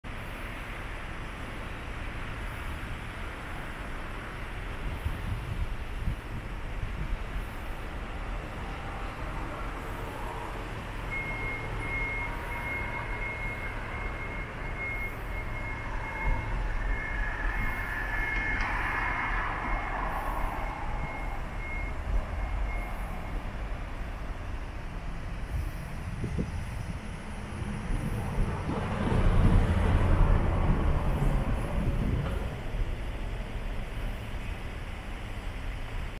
【駅前シリーズ】 近鉄志摩線 鵜方駅 Kintetsu Shima Line Ugata Station (2020.12)
三重県志摩市、近鉄志摩線の鵜方駅。
2020年12月、DJI Osmo Pocketで撮影。

Mie Japan railways trains
近畿日本鉄道　三重県志摩市
긴테쓰 시마선　우가타역